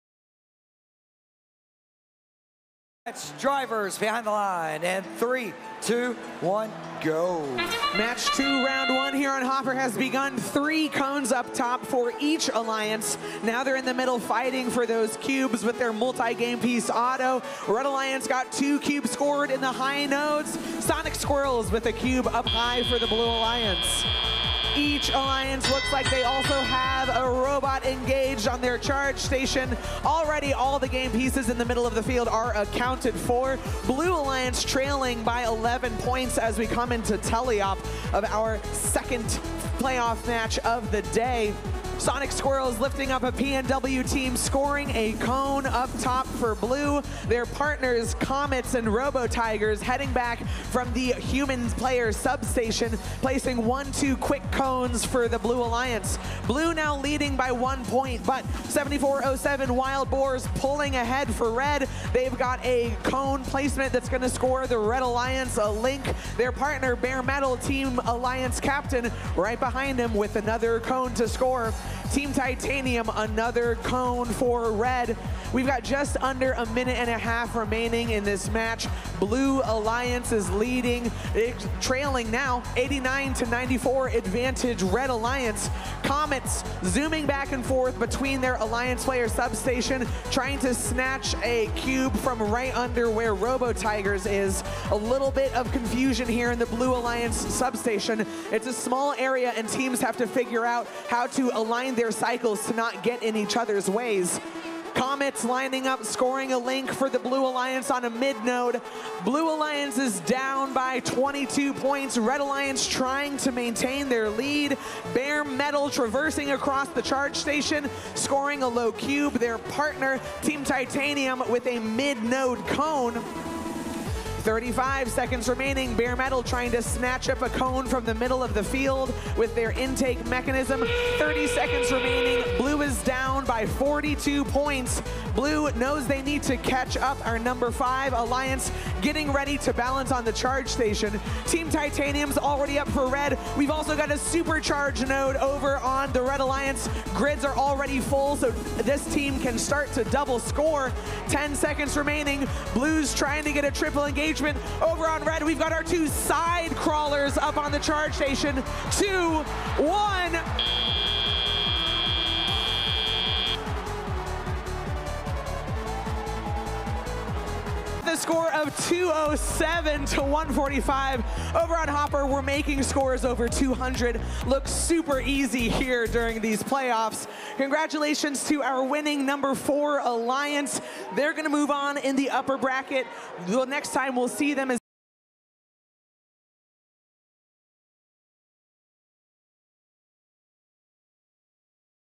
Match 2 (R1) - 2023 FIRST Championship - Hopper Division
Match 2 (R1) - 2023 FIRST Championship - FIRST Robotics Competition - Hopper Division